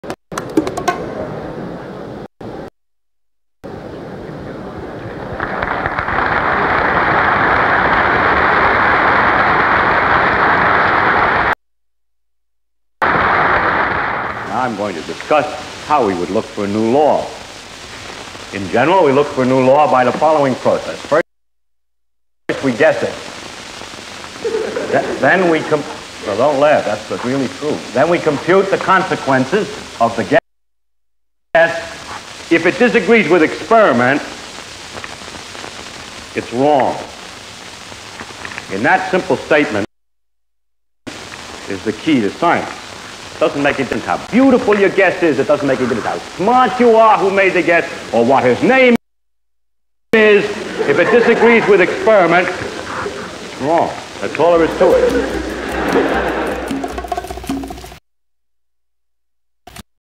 Feynman on arguments from authority
World famous Nobel Prize winner Professor RIchard Feynman gives a classic explanation of how a good scientist treats authority and evidence